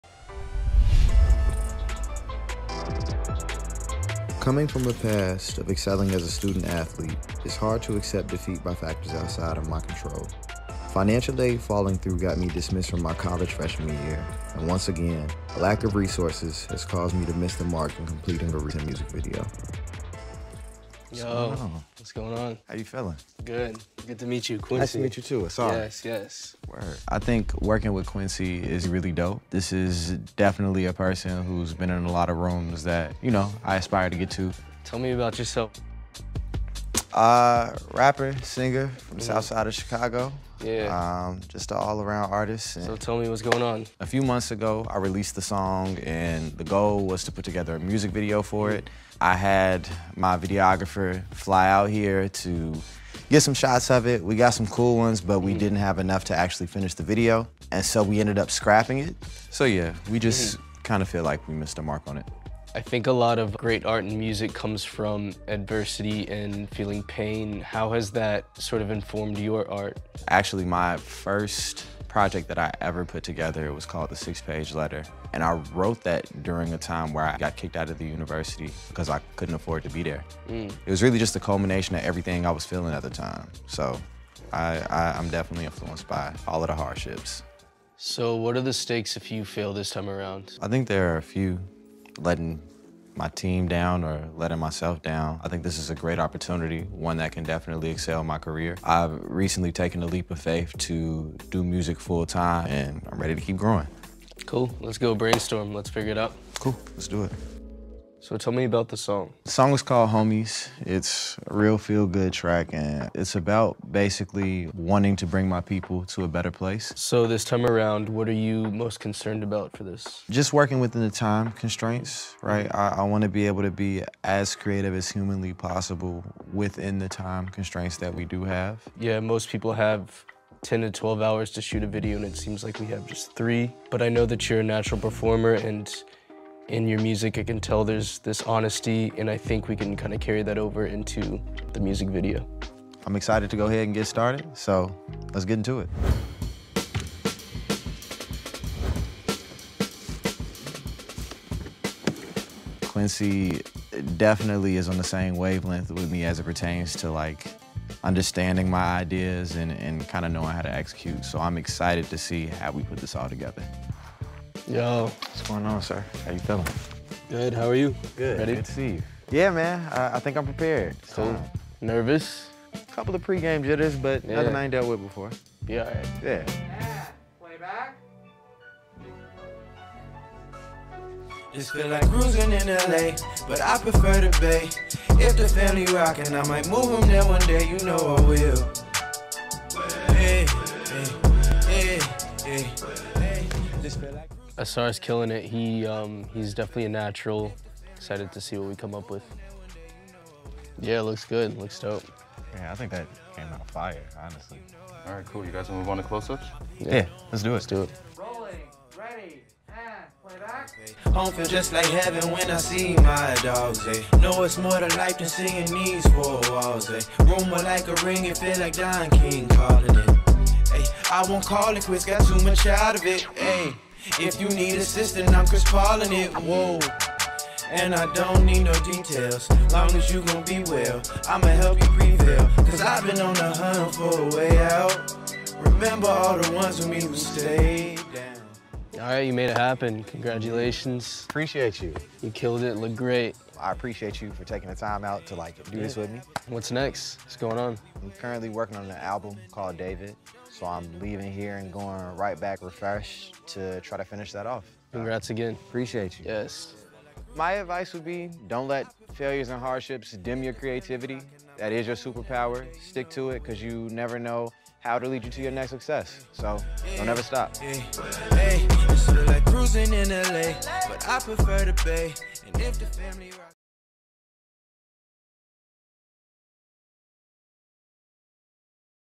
Next Success series w/ AUSAR: presented by UPROXX & Sprite
In a new video from the Next Success series (presented by Uproxx and Sprite), Ausar gets the chance to work with Quincy Banks, an established creative pro in Los Angeles, who's contributed key visual elements to projects by artists like Lizzo, The Rolling Stones, and Charli XCX, among plenty of others.